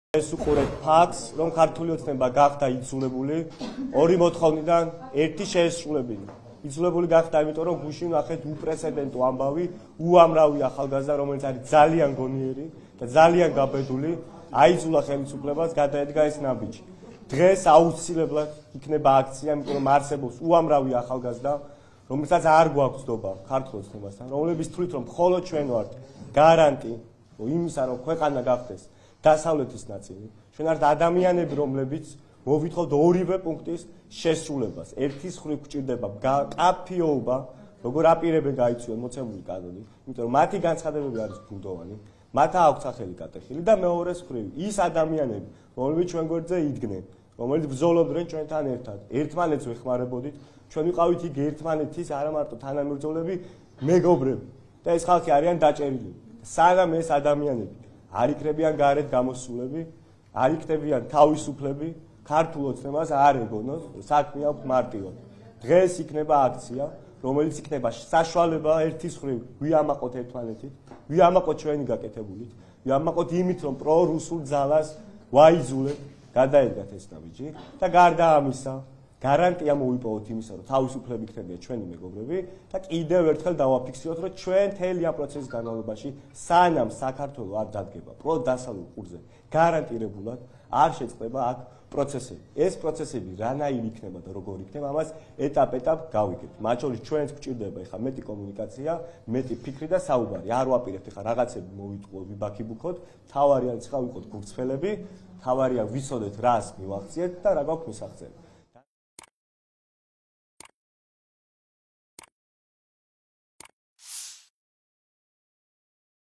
9 მარტს, პარლამენტთან, 19:00 საათზე აქცია გაიმართება
▪️ „დღეს იქნება აქცია, რომელიც იქნება საშუალება, ვიამაყოთ ერთმანეთით, ჩვენი გაკეთებულით, ვიამაყოთ იმით, რომ პრორუსულ ძალას ვაიძულეთ, გადაედგათ ეს ნაბიჯი და გარდა ამისა, გარანტია მოვიპოვოთ“ -  ცოტნე კობერიძე